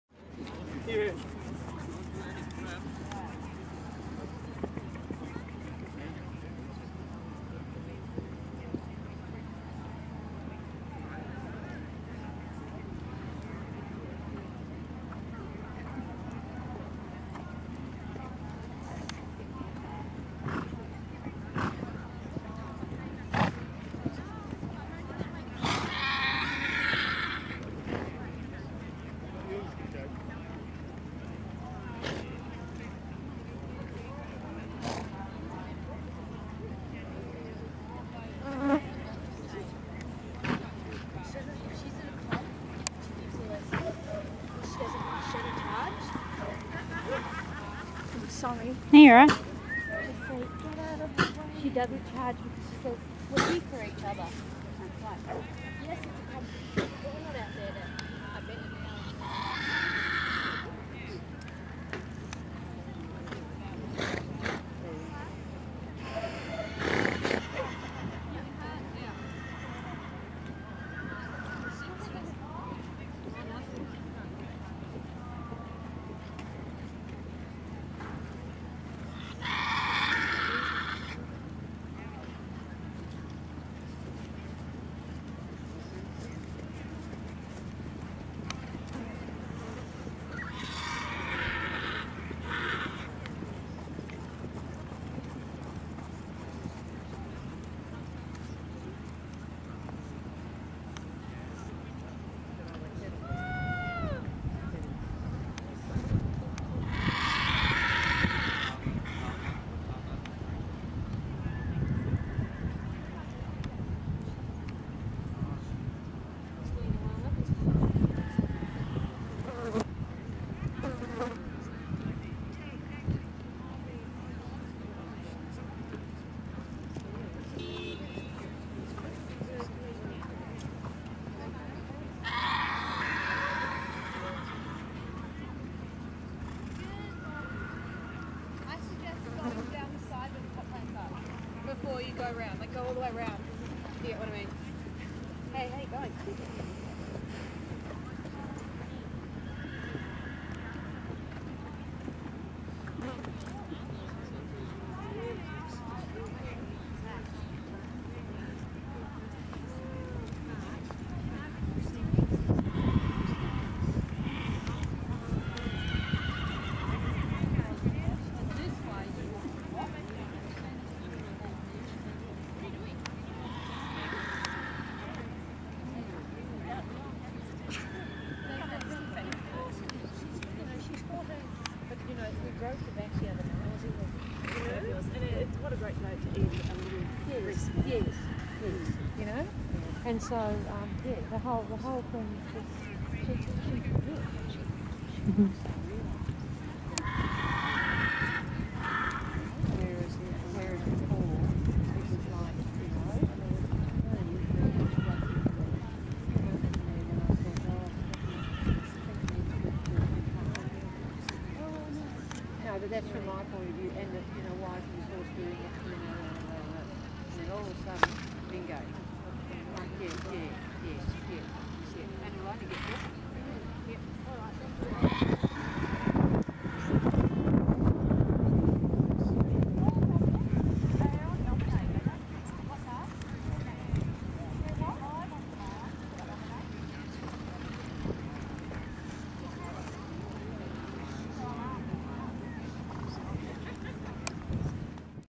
Safari Dressage L&C 2012
Safari dressage at Lakes and Craters 3Day Event 2012 Preliminary